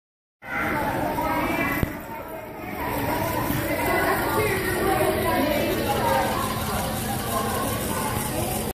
Slow and Steady with a Push from Behind
Making robots move without the use of wheels or anything too round!